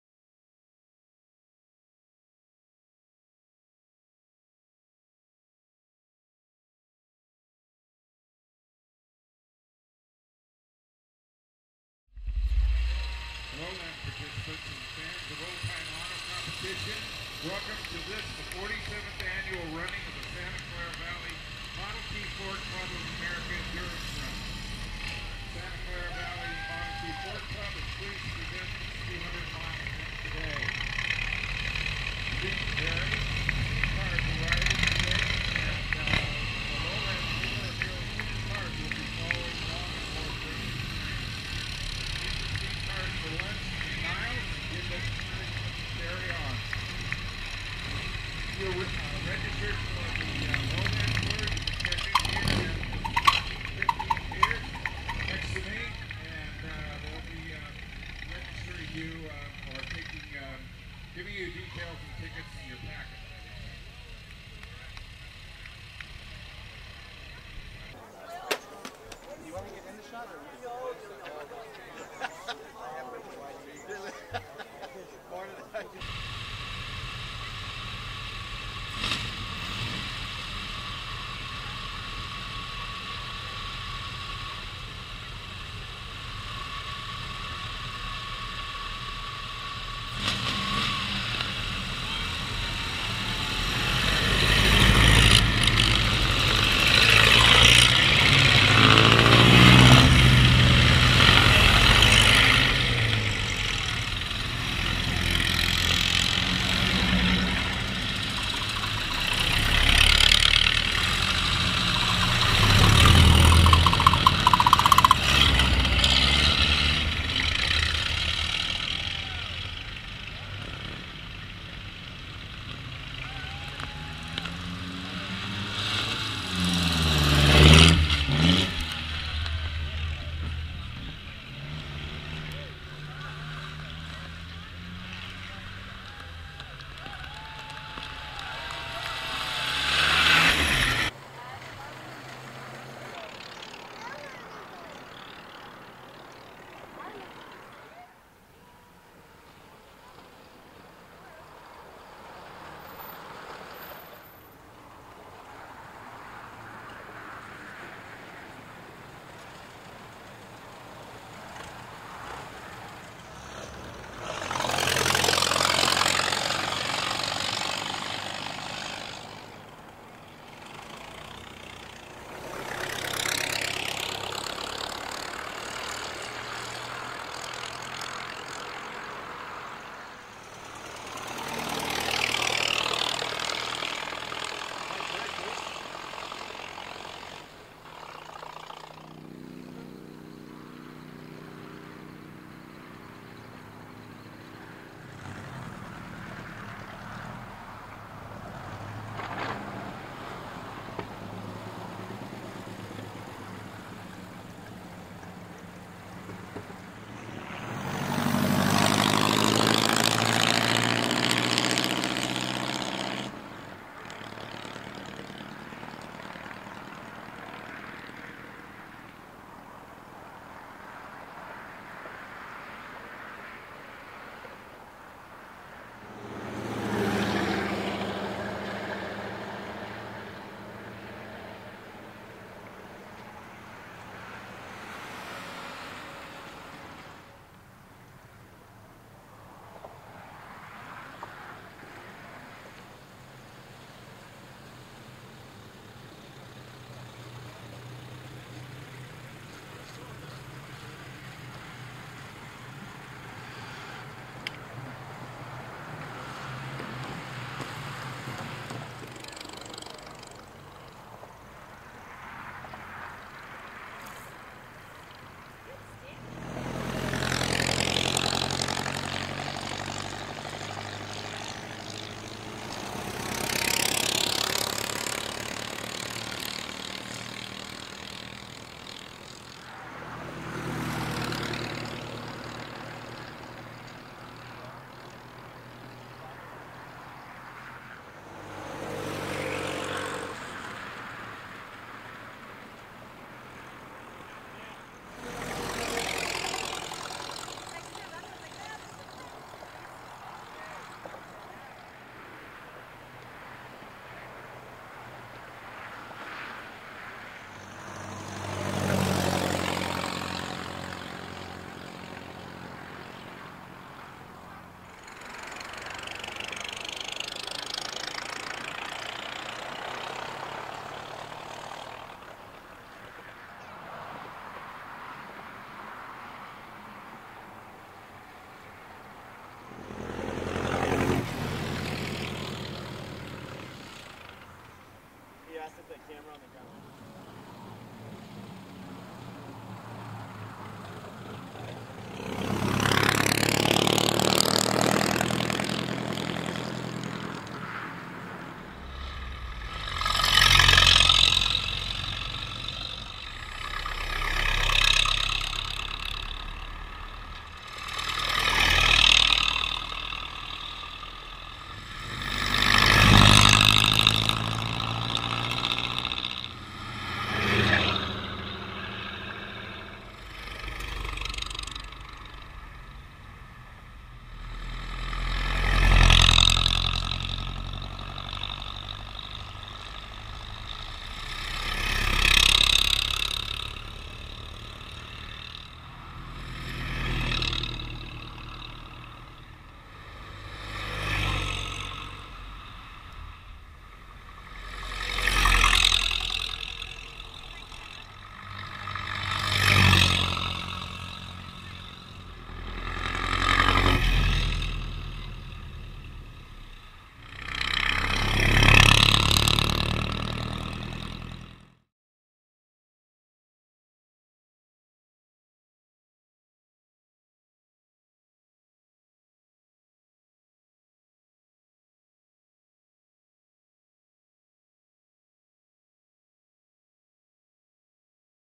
2016 SCVMTFC Endurance Run
47th Annual Endurance Run and Lowland Tour hosted by the Santa Clara Valley Model T Ford Club (SCVMTFC).